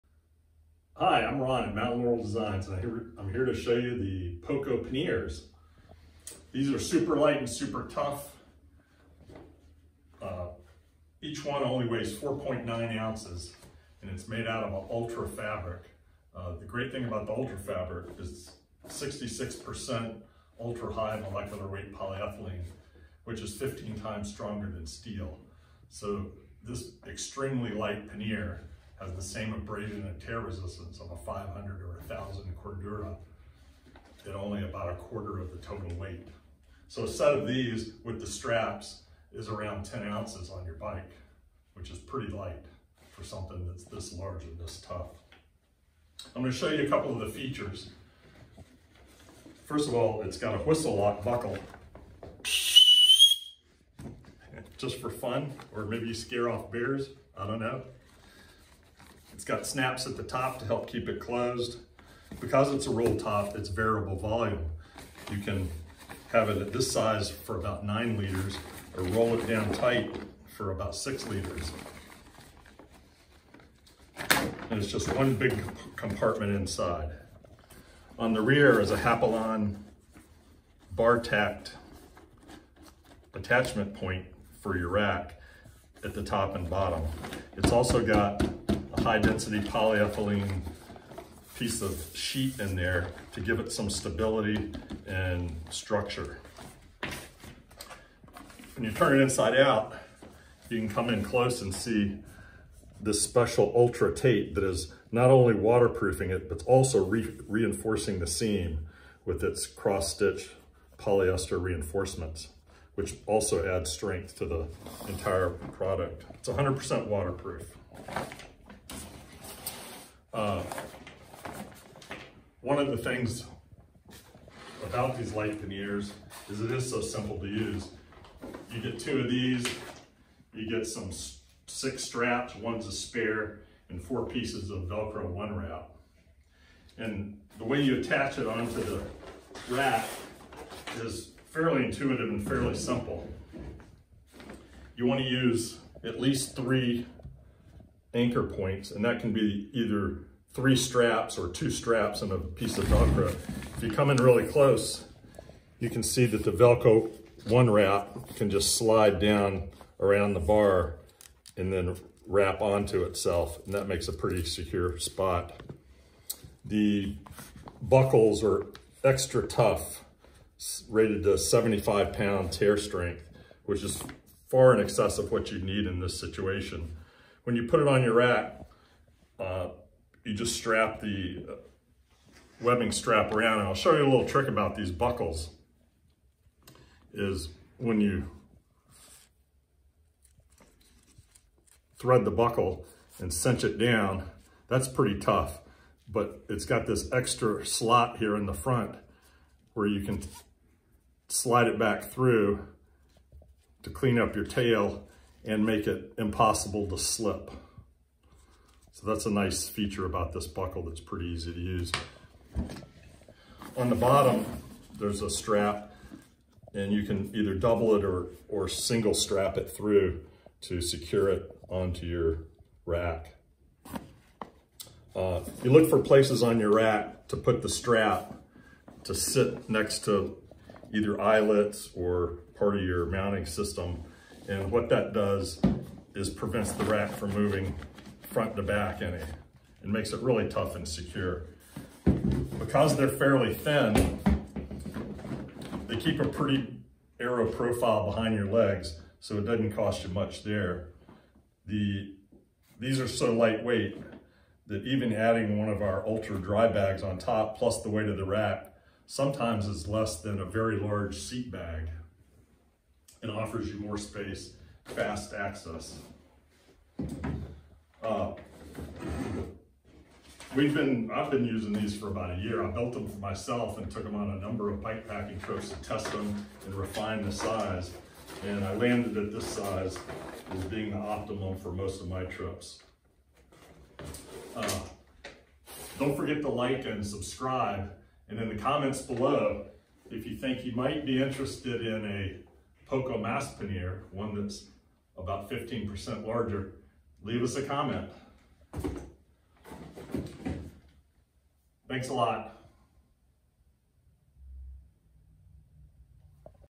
MLD ULTRA POCO PANNIERS
We have Launched The V2 Poco Panniers. Please See That Video.  They are similar to the V1, so we have left this video up for now in case any previous users need to reference this video. Ulta Poco Panniers  4.9oz ea.  Lightest Tough Small Panniers Available.  BikePacking Light!
Fits almost all types of bike racks. Waterproof.
Built Tough from Challege EcoPak Ultra 200 UHWMPE Fabric.